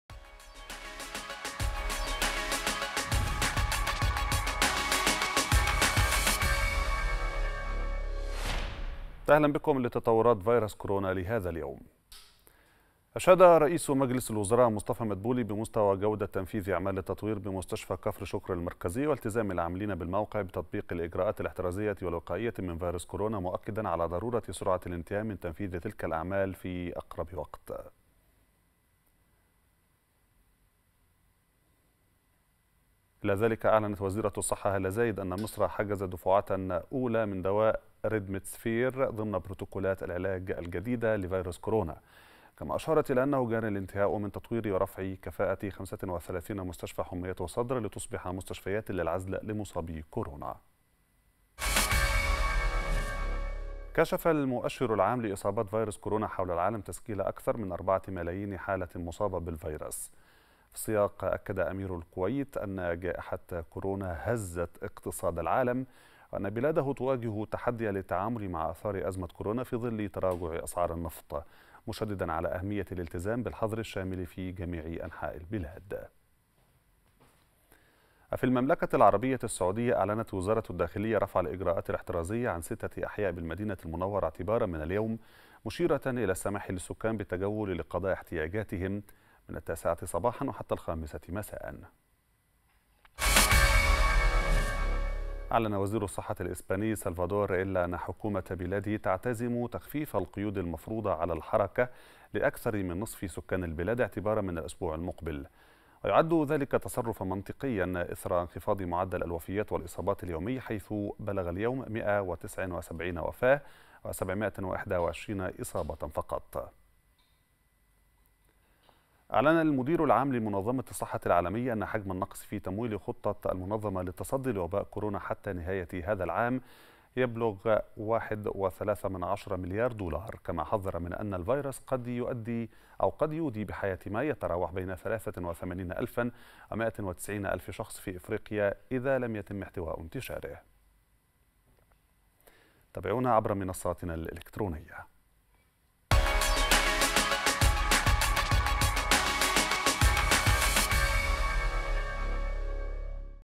أخبار كورونا | وزيرة الصحة تعلن حجز دفعة من دواء "ريمديسيفير" للعلاج من كورونا
قالت الدكتورة هالة زايد وزيرة الصحة والسكان، إن مصر حجزت دفعة أولى من دواء "ريمديسيفير" والذي تم اعتماده من منظمة الغذاء والدواء الأمريكية ضمن بروتوكولات العلاج الجديدة لفيروس كورونا المستجد.
وأضافت خلال جولتها بمستشفى حميات الإسكندرية اليوم السبت، أنه يتم متابعة بروتوكولات العلاج المحدثة بشكل مستمر مع منظمة الصحة العالمية، موضحة أنه يتم استخدام بلازما المتعافين لعلاج المصابين بفيروس كورونا، وسيتم الإعلان عن النتائج أولًا بأول.